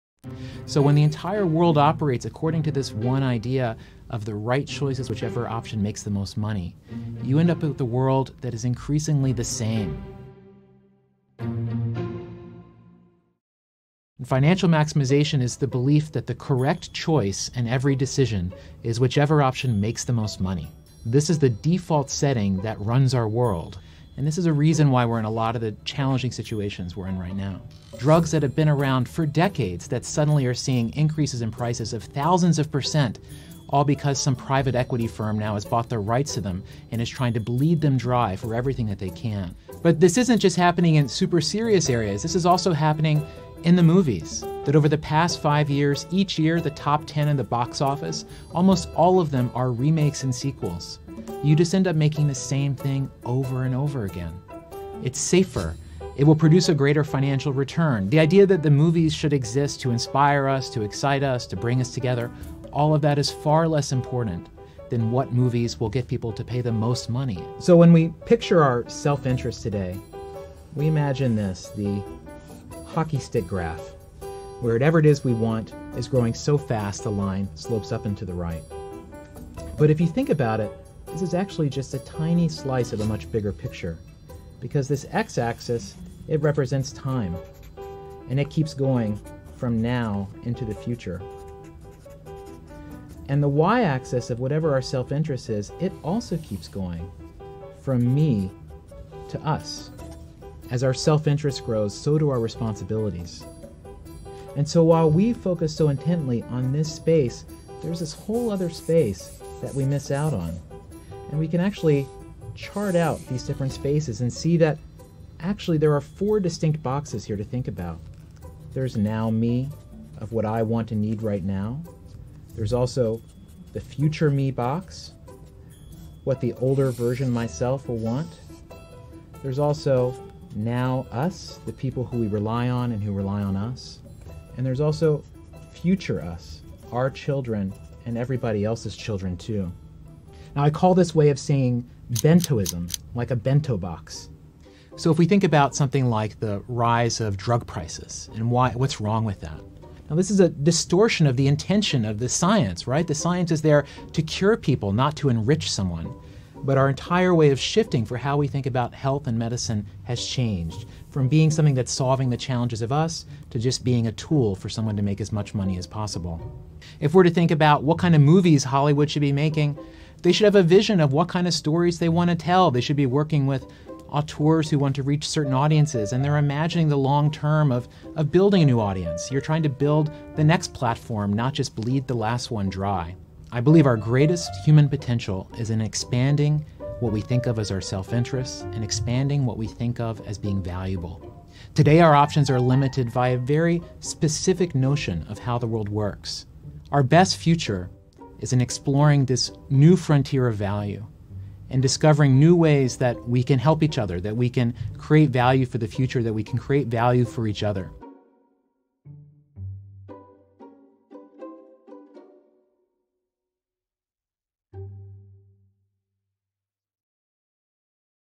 Bentoism: A post-capitalist vision of the future
A video made with NBC News explaining Bentoism and my book "This Could Be Our Future: A Manifesto for a More Generous World"